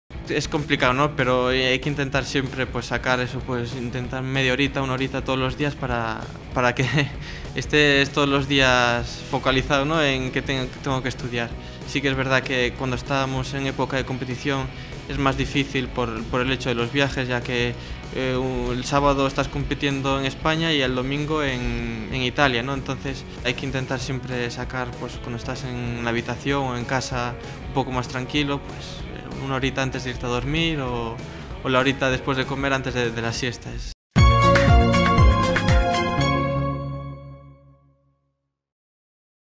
Hablamos de... Como compatibilizar estudios y deporte
¿Te interesa la nutrición? ¡Infórmate sobre nuestro Grado Oficial en Nutrición Humana y Dietética (NHyD)

Antonio Serrat, triatleta y alumno del Grado Oficial en Nutrición de la Universidad Isabel I y del Programa UNARD, nos cuenta cómo se organiza para compatibilizar el deporte de élite con su formación.